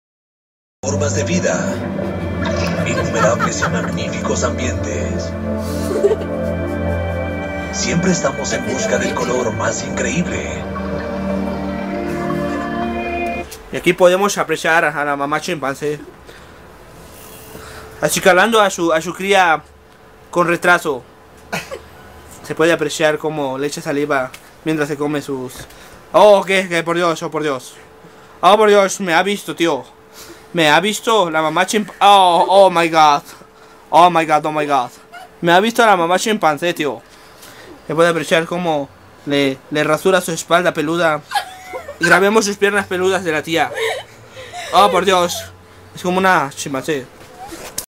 mamá chimpancé
Estabamos en el cuarto, cuando de repente. . . volteamos y había una mamá chimpancé acicalando a su cría con retraso XD XD X333